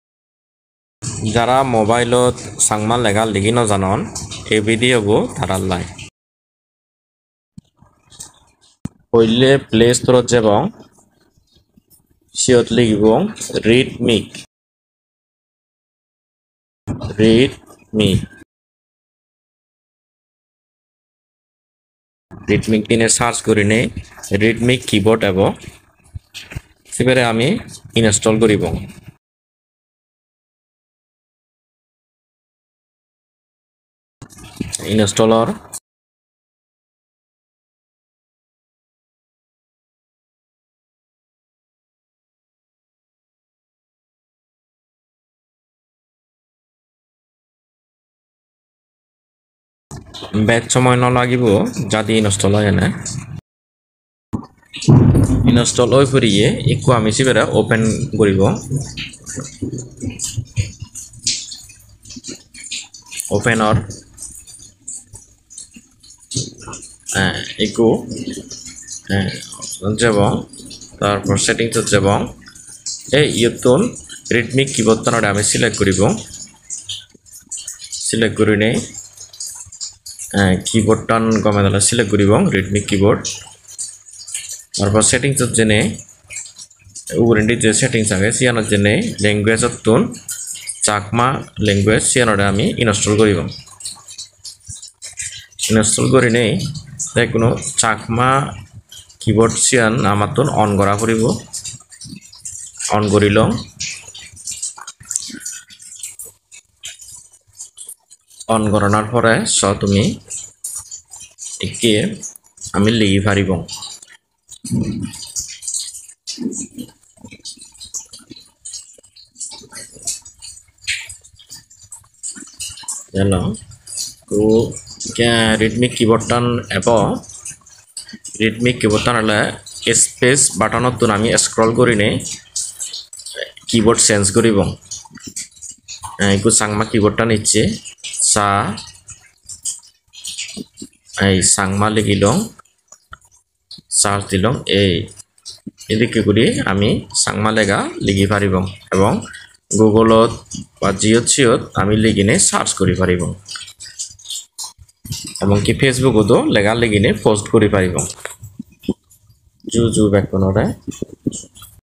How to install chakma keyboard || চাকমা কীবোর্ড
How to install chakma keyboard with ridmik keyboard app